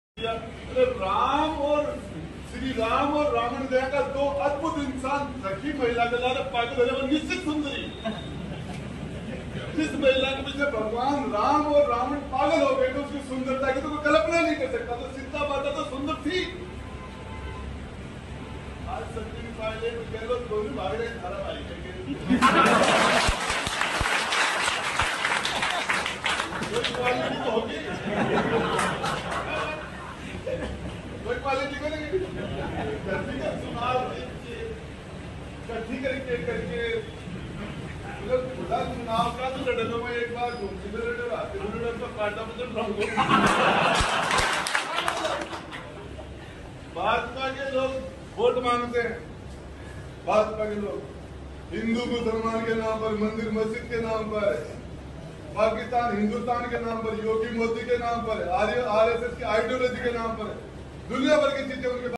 Rajasthan News:'राम और रावण सीता की सुंदरता के पीछे पागल थे'-राजस्थान Congress विधायक Rajendra Gudha
गहलोत सरकार में मंत्री राजेंद्र गुढ़ा ने पहली बार विवादित बयान नहीं दिया है। इससे पहले भी कई बार अपने ऐसे ही बयानों से वो चर्चा में रहे हैं। बीते दिनों राजेंद्र गुढ़ा ने अपने विधानसभा क्षेत्र के अफसरों से कहा था कि उनके इलाके में सड़कों को कटरीना कैफ के गालों को तरह बनाना होगा। और भी विवादित बयान वो दे चुके हैं।

Minister Rajendra Gudha in the Gehlot government has not given a controversial statement for the first time. Even before this, he has been in discussion many times with his similar statements. In the past, Rajendra Gudha had told the officers of his constituency that the roads in his area would have to be made like the cheeks of Katrina Kaif. He has also given more controversial statements.